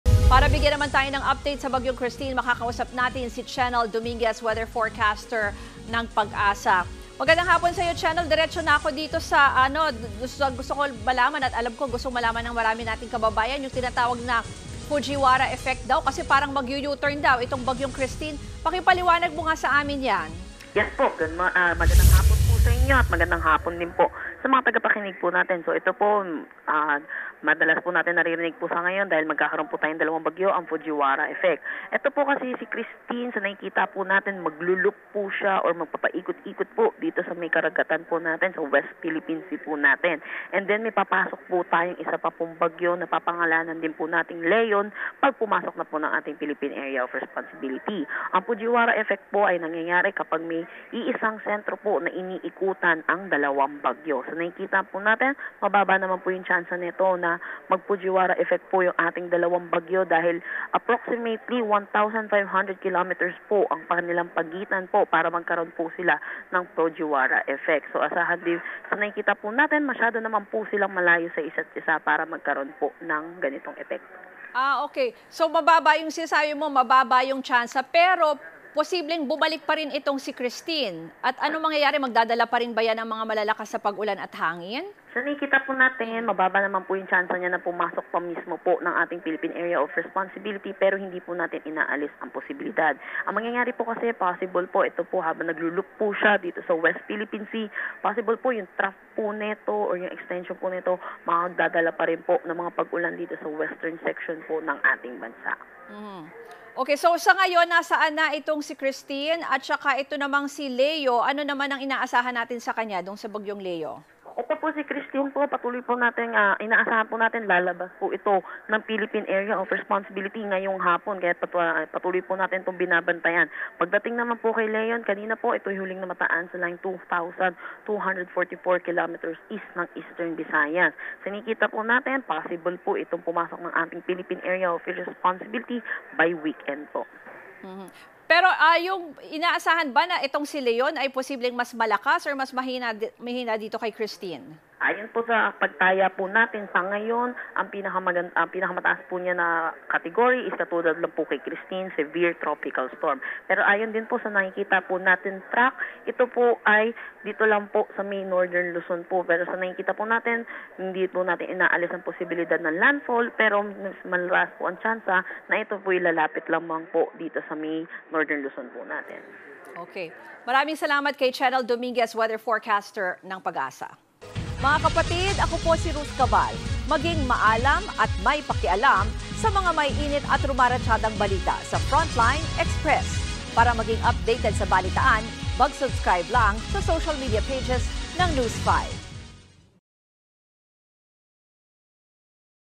Bagyong Leon, posibleng pumasok ng PAR at hatakin pabalik ang Bagyong Kristine I Frontline Express
FrontlineExpress I Nakapanayam ng News5 si PAGASA weather forecaster Chenel Dominguez para magbigay ng iba pang detalye hinggil sa Bagyong Kristine.

Binabantayan rin ng PAGASA ang isang tropical depression na papangalanang LeonPH kapag pumasok na ng PAR.